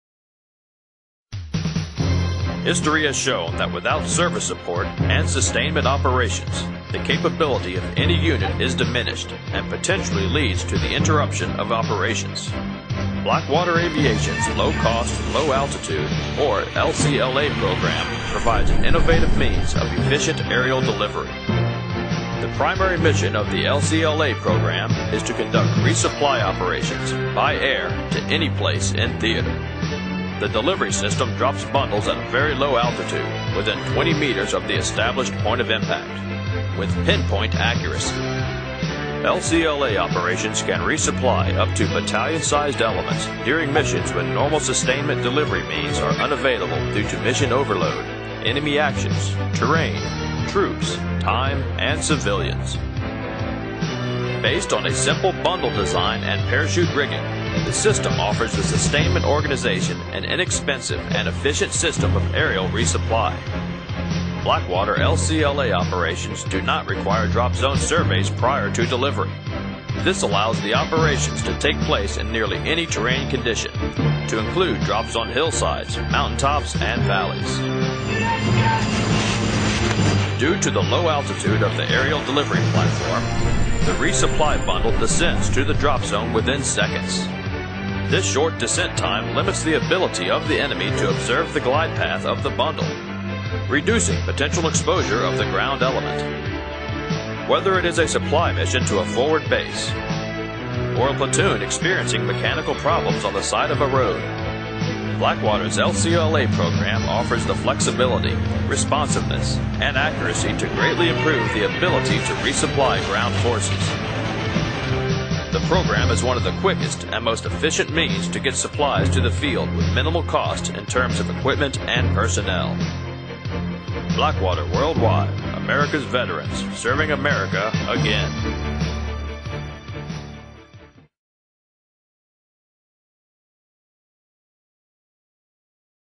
Ex-Blackwater LCLA
Blackwater Low Cost Low Altitude provides air support of ammunition, food, and supplies.